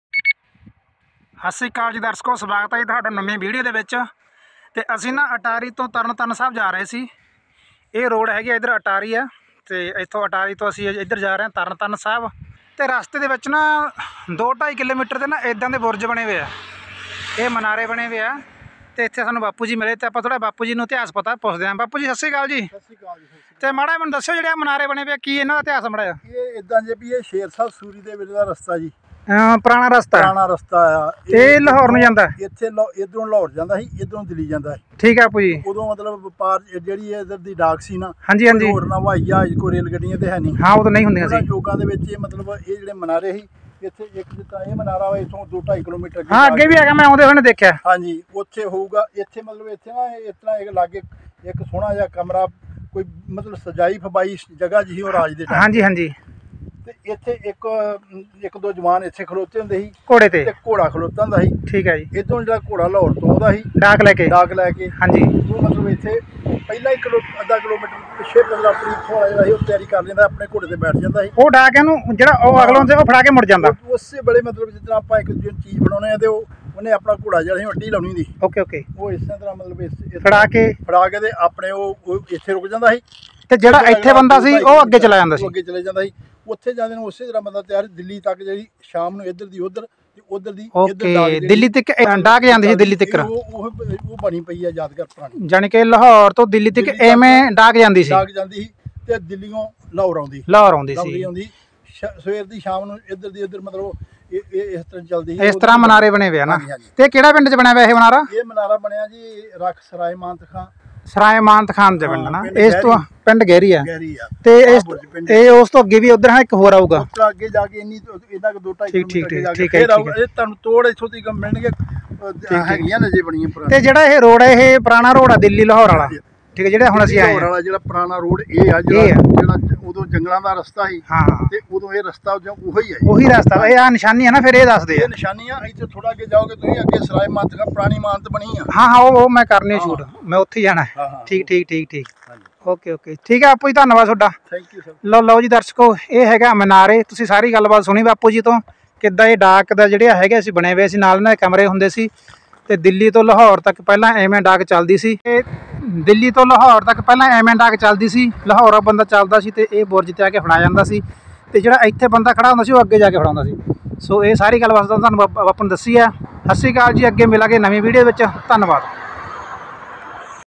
ਪੁਰਾਣੇ ਸਮਿਆਂ ਵਿੱਚ ਲਾਹੌਰ ਤੋਂ ਦਿੱਲੀ ਤੱਕ ਇਸ ਤਰ੍ਹਾਂ ਚਲਦੀ ਸੀ ਡਾਕ Resham video junction
punjab punjab
My content is historical place in punjab (india) World ALL